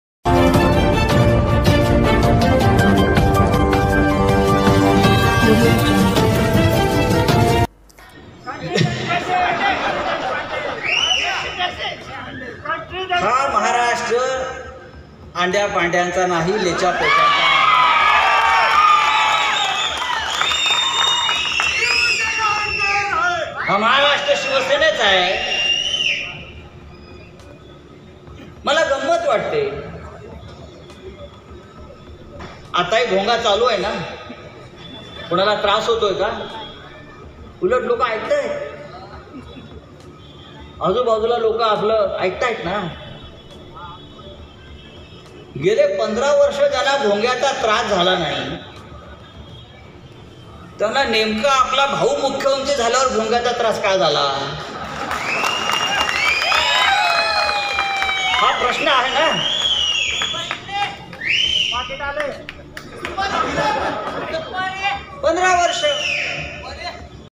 हा महाराष्ट्र अंड्या पांड्यांचा नाही,शिवसेनेचा आहे भाऊ मुख्यमंत्री झाल्यावर भोग्यांचा त्रास का झाला:
हा महाराष्ट्र अंड्या पांड्यांचा नाही,शिवसेनेचा आहे भाऊ मुख्यमंत्री झाल्यावर भोग्यांचा त्रास का झाला: संजय राऊत